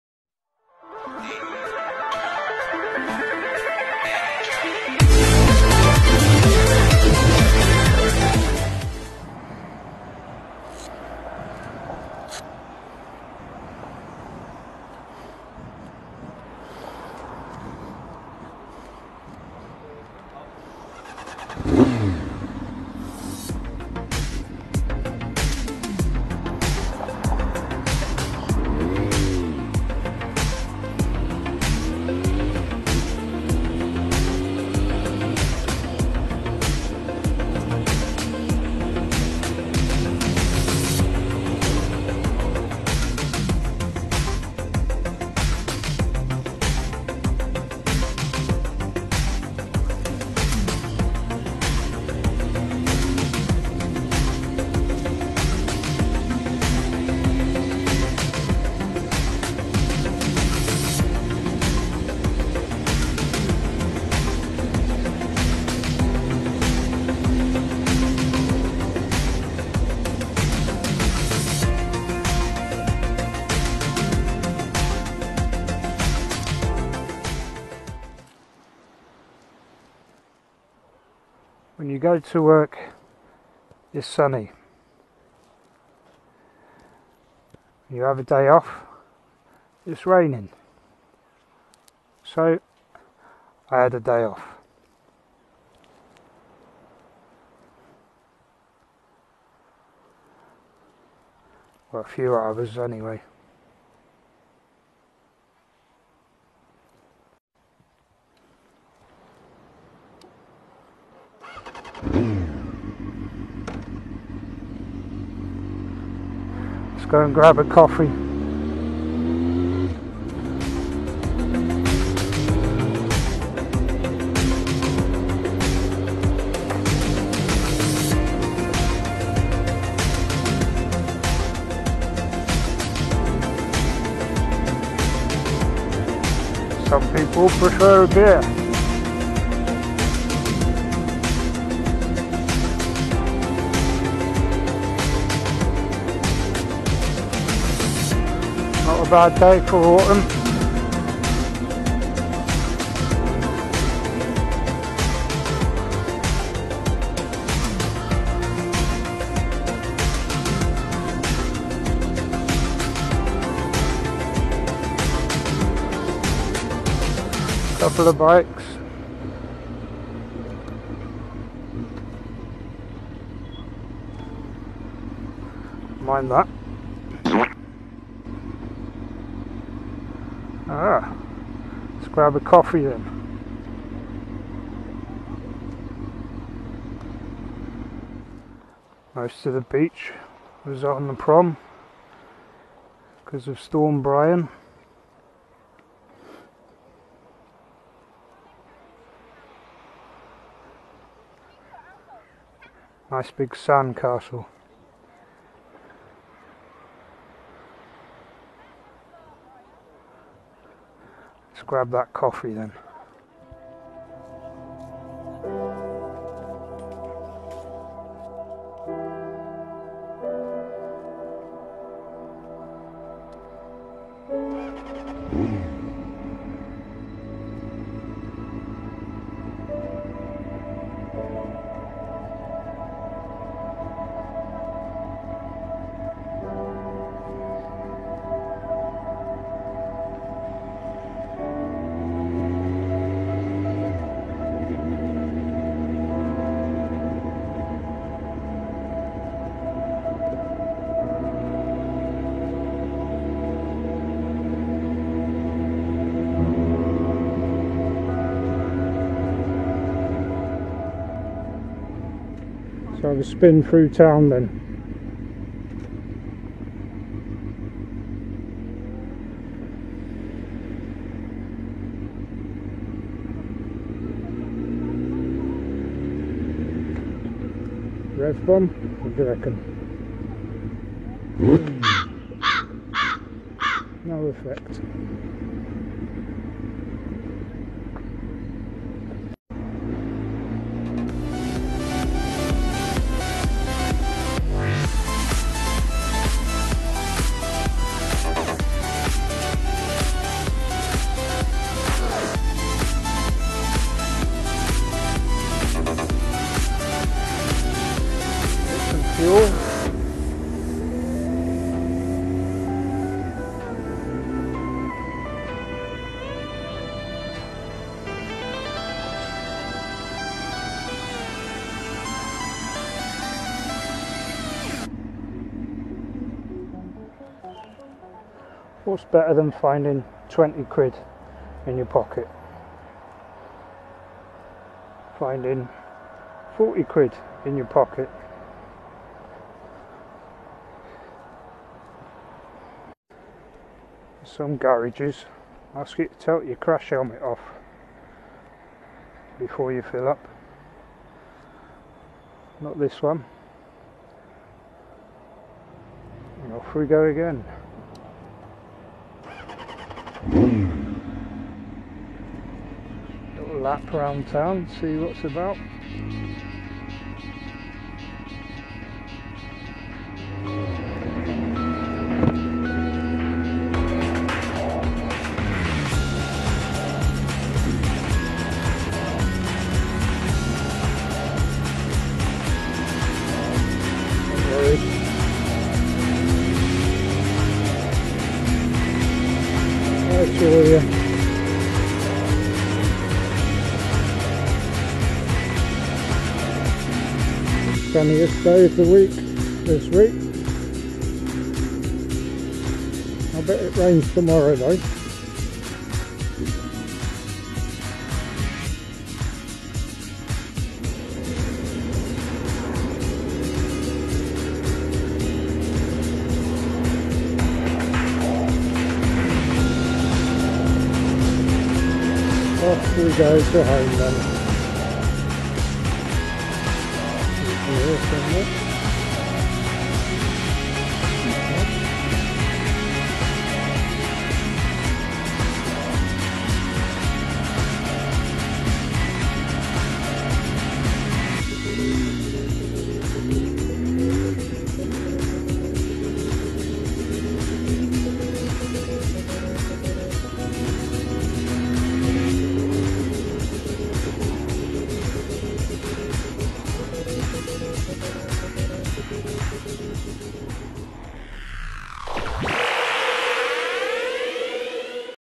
Couple hours in the sun
Managed to get a short ride out in the sun & a coffee